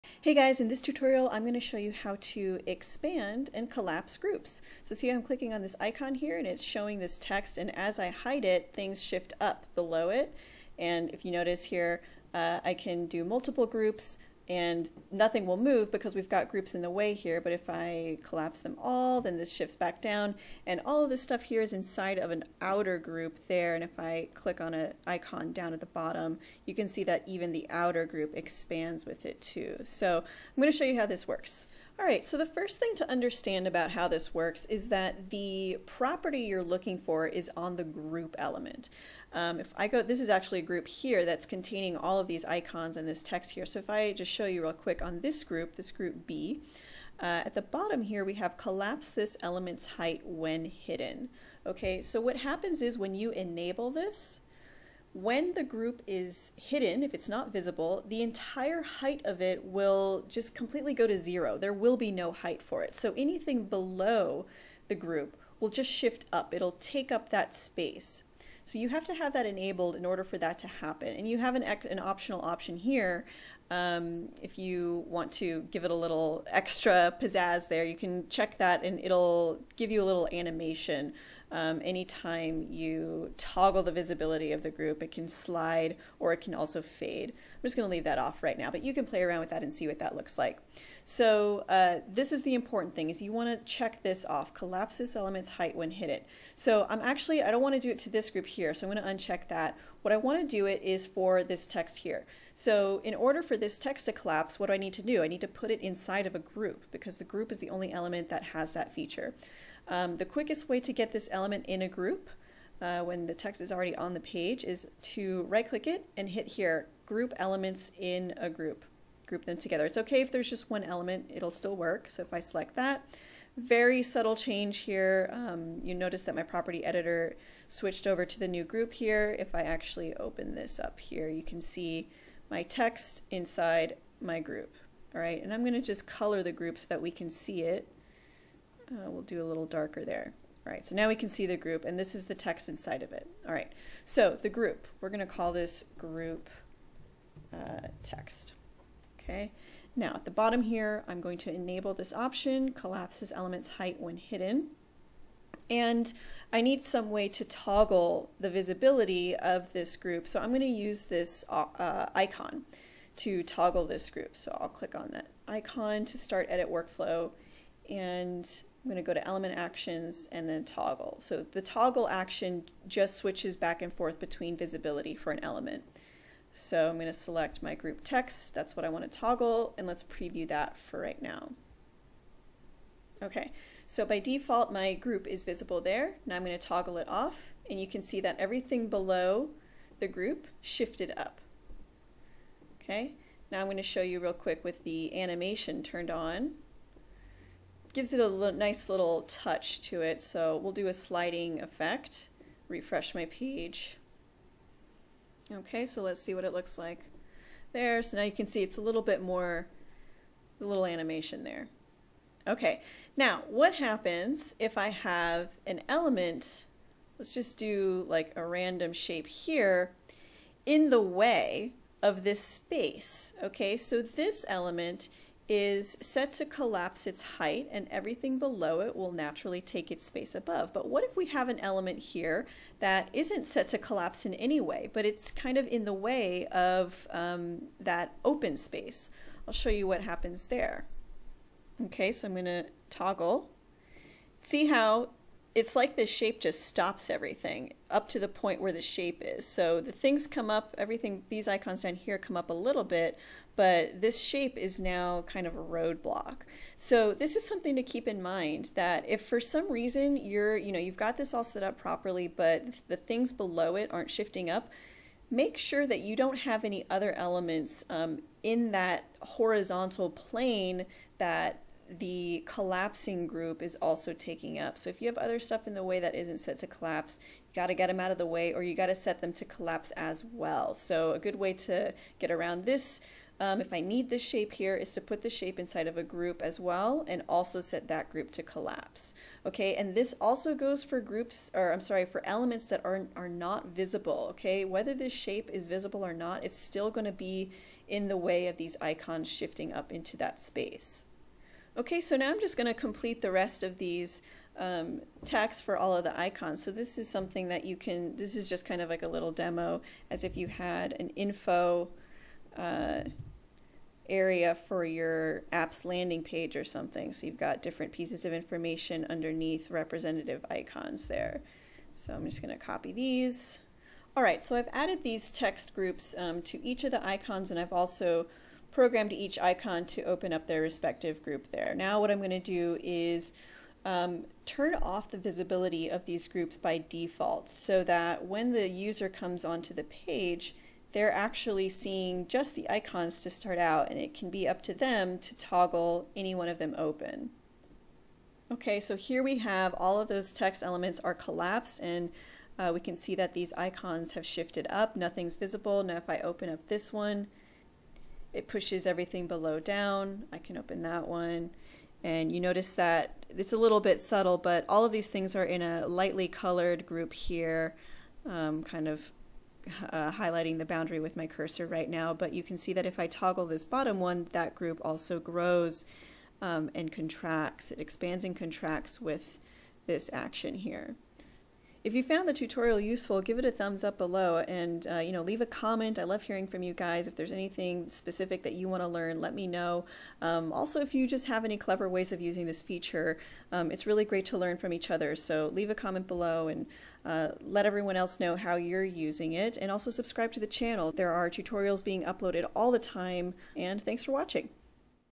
Working With Groups in Bubble.io (How to Collapse and Expand)
In this Bubble.io lesson, learn how to collapse and expand groups in your app. This is a common functionality that's helpful to understand.

Your app's design is one of the most important aspects to users. While the functionality and features are vital to its success, your users will consciously (and subconsciously) either stick around long term or find another solution if the design, organization, and overall user ease of your Bubble app isn't top notch.

There are tons of simple tricks to create a smoother flow through your app, and most of them are easy to implement if you know how.

Take collapsible groups for example. Your app probably has a lot of information to share with users. If you include every single word, number, or line of text in large chunks throughout your pages, things can appear cluttered and hard to navigate.

A simple trick is to make your groups collapsible. In other words, users can see a small blurb or even an image and choose to click and reveal more information.

By doing this, you can keep your app clean, clear, and free of clutter, giving users a visually pleasing experience (and a lot less overwhelm).

Use this tutorial to create collapsible groups in your own no code app.